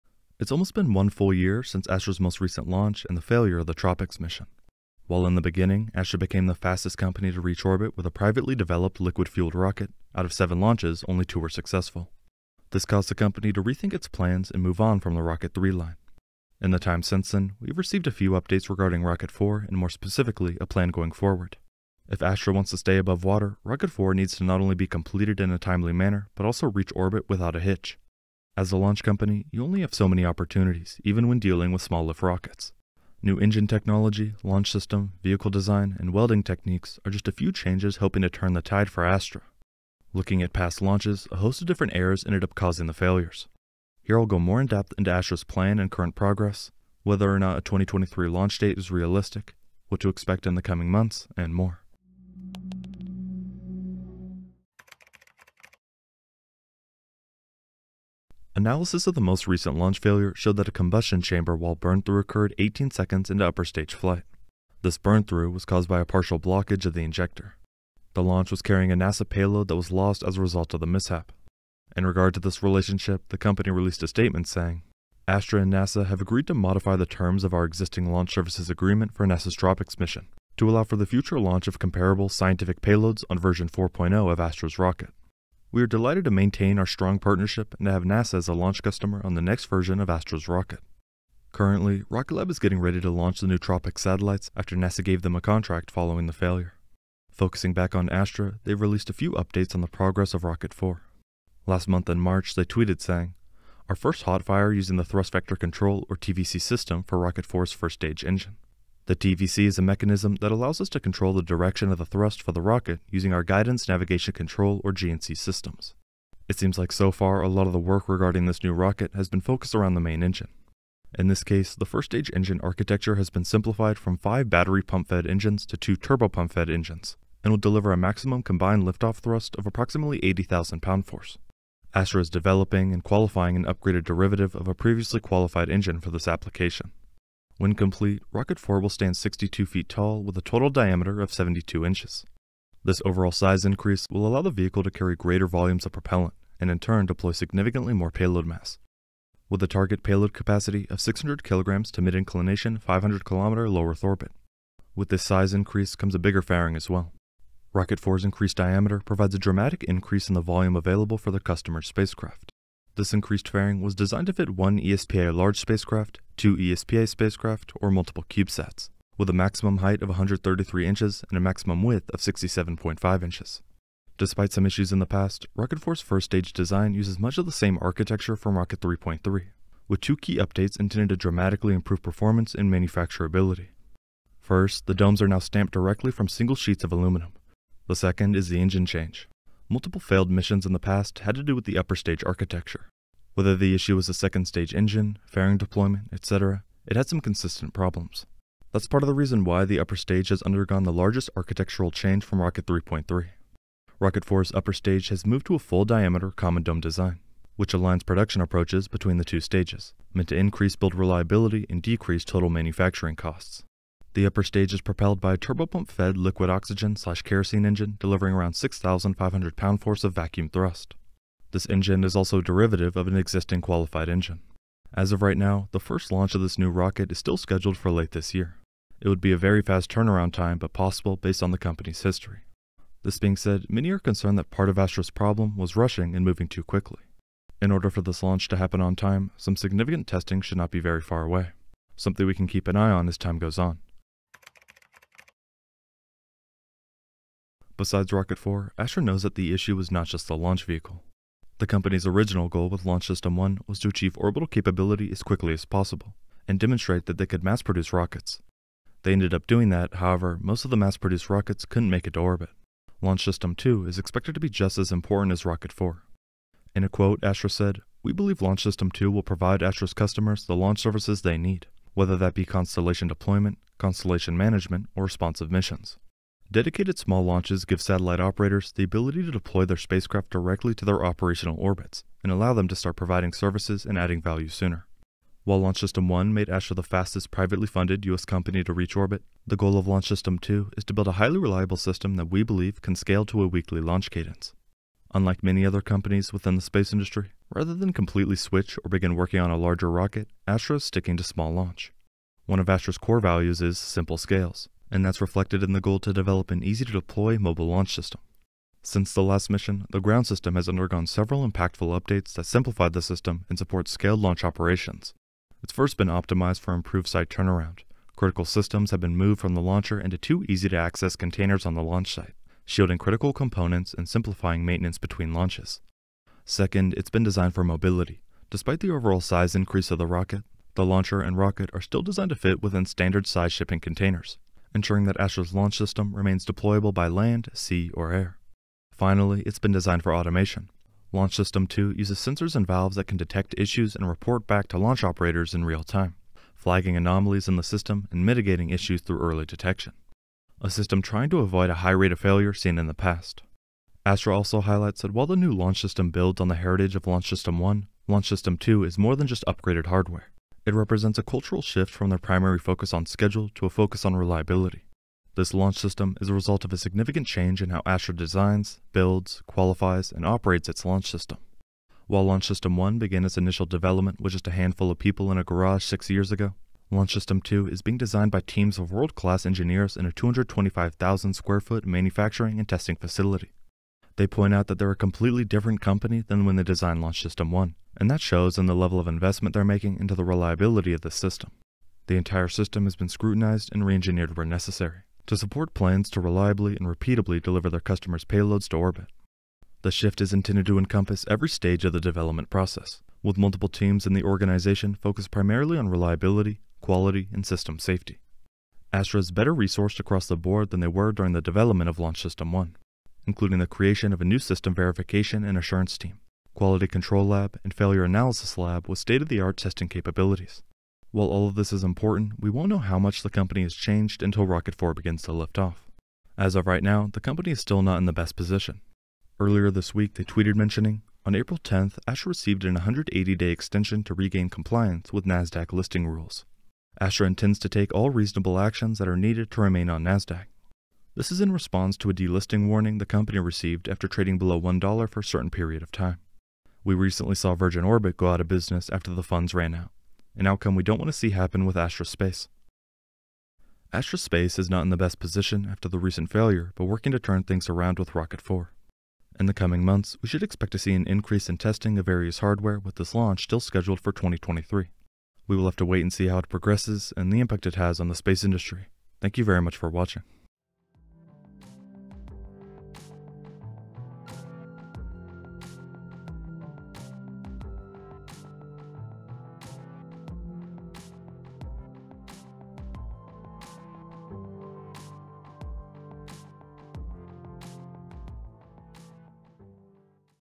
What Is Astra's Plan To Stay In Business?
It’s almost been one full year since Astra’s most recent launch and the failure of the TROPICS mission. While in the beginning, Astra became the fastest company to reach orbit with a privately developed liquid-fueled rocket, out of 7 launches only two were successful. This caused the company to rethink its plans and move on from the Rocket 3 line.

In the time since then, we have received a few updates regarding Rocket 4 and more specifically a plan going forward. If Astra wants to stay above water Rocket 4 needs to not only be completed in a timely manner but also reach orbit without a hitch. As a launch company, you only have so many opportunities even when dealing with small-lift rockets.

New engine technology, launch system, vehicle design, and welding process are just a few changes hoping to turn the tide for Astra. Looking at past launches, a host of different errors ended up causing the failures. Here I will go more in-depth into Astra’s plan and current progress, whether or not a 2023 launch date is realistic, what to expect in the coming months, and more.

Chapters:
0:00 - Intro
0:54 - Astra's Current Progress
4:04 - Not Just The Rocket